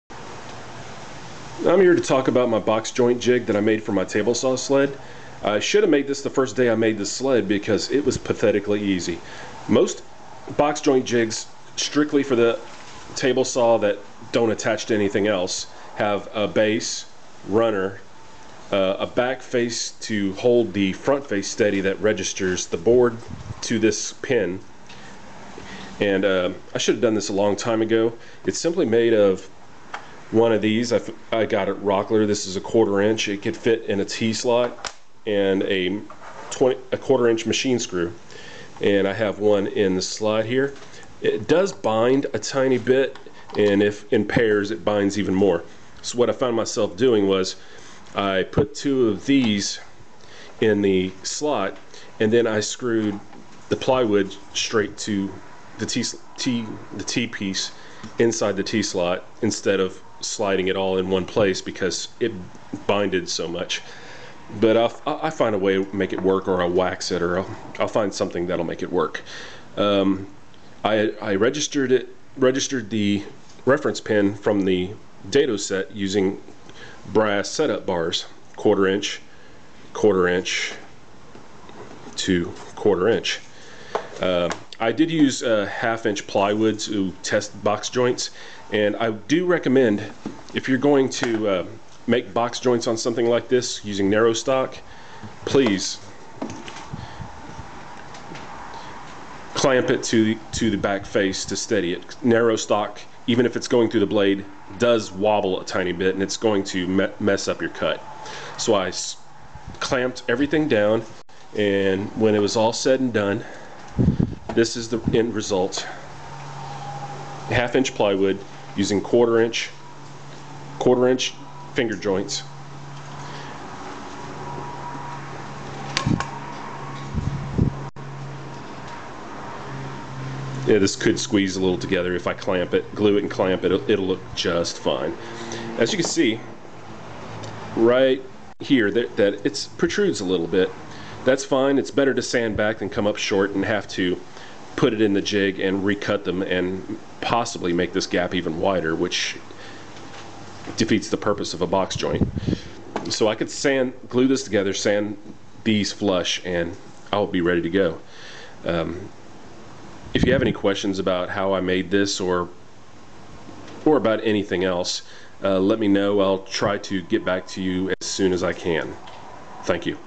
Box joint jig for the table saw sled
Finally took the time to make a box joint jig that works with my adjustable table saw sled.  The jig itself consists of one 1/2"  plywood back, two 1/4" machine screws, and two 1/4" threaded t-slot runners.
The screws sit within a counter bore in the plywood  so the screws head is below the plywood's face.  A 1/4" dado stack was installed in the saw and a 1/4"x1/4" dado was cut in the face for the 1/4" square stock that would become the referencing pin.  The reference pin was moved to the right and I placed a 1/4" brass setup bar between the reference pin and the edge of the blade's teeth to establish what will be the fingers of the joint.